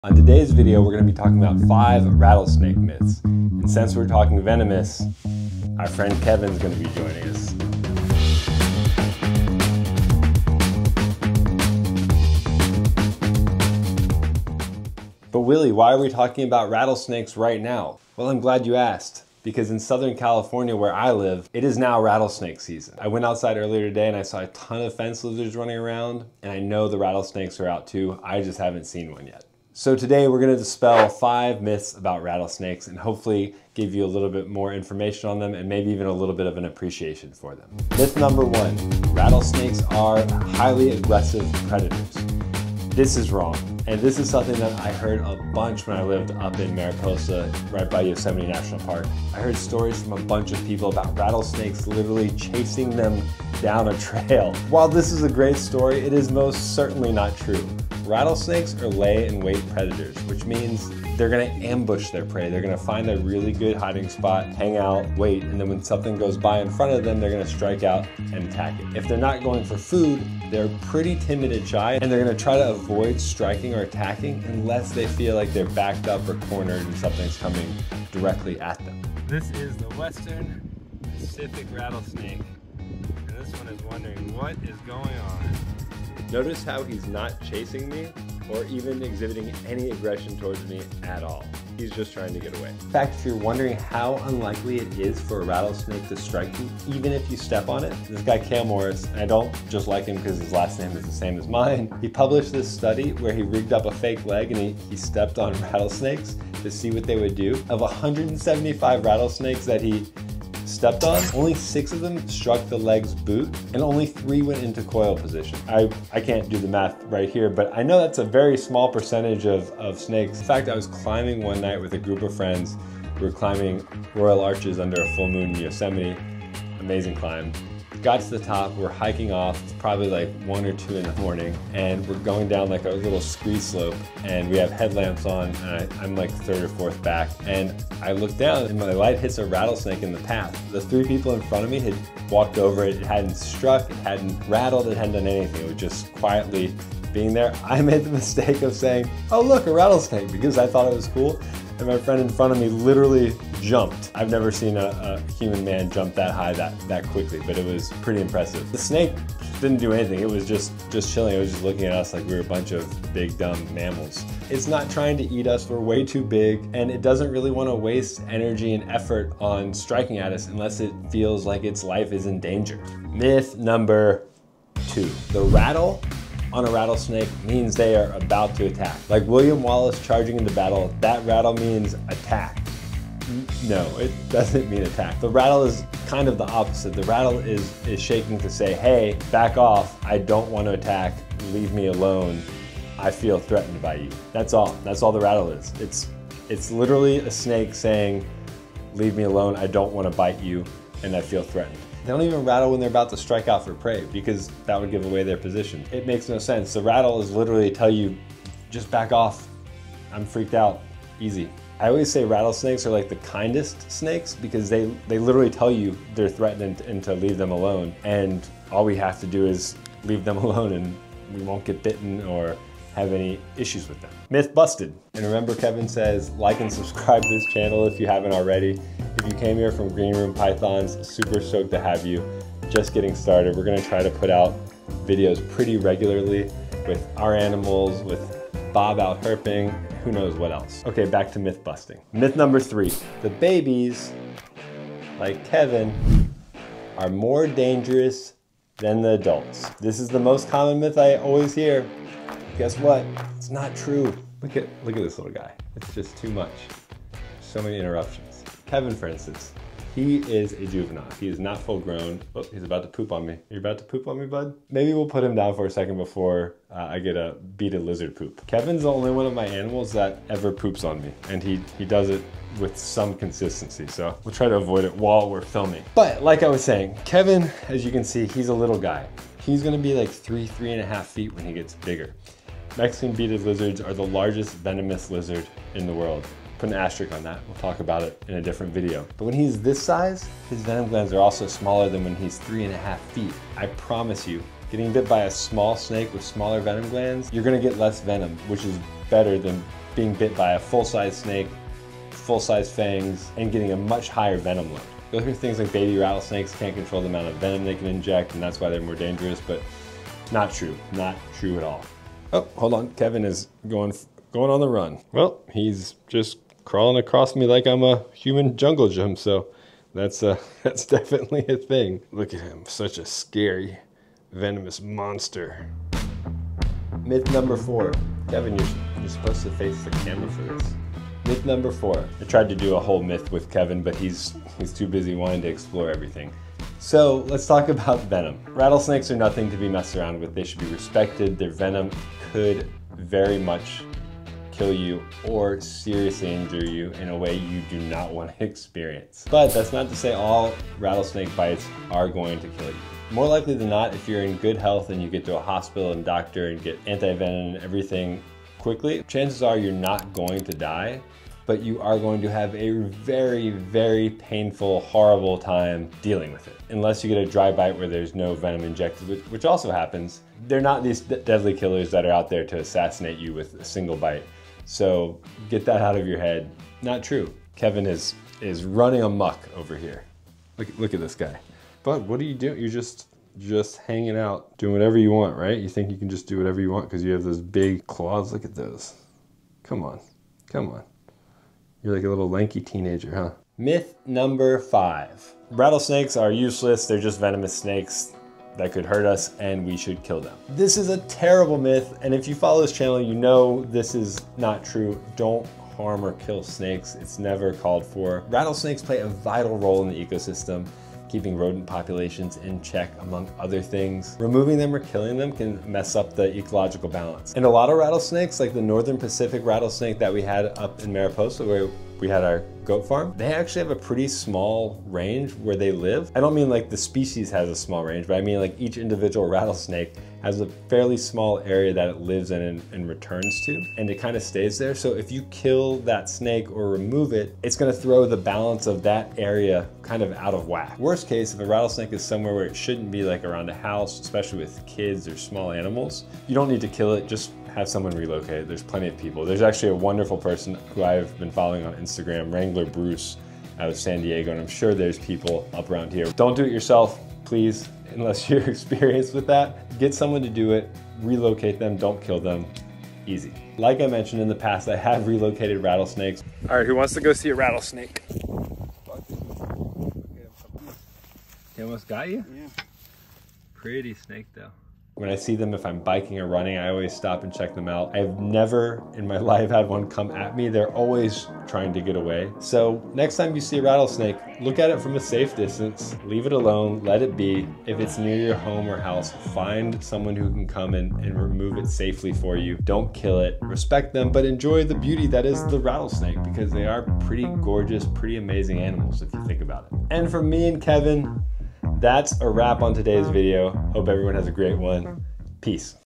5 Rattlesnake Myths You Still Believe 🐍 BUSTED!
Think you know rattlesnakes? 🐍 Think again.

In this video, I’m busting 5 of the most common myths people still believe about rattlesnakes — including whether they chase people, how dangerous babies really are, and what that rattle actually means.

We're in the middle of rattlesnake season here in Southern California, so it’s the perfect time to set the record straight. And of course, my venomous buddy Kevin joins in to help.

✅ What You’ll Learn:
Are rattlesnakes actually aggressive?

Does the rattle mean they’re about to strike?

Are baby rattlesnakes more dangerous?

How deadly are their bites really?

Should we be killing them, or protecting them?

Whether you're a herper, hiker, or just curious, this video will give you a new appreciation for one of nature’s most misunderstood animals.

📍 Seen a rattlesnake lately? Drop your story in the comments!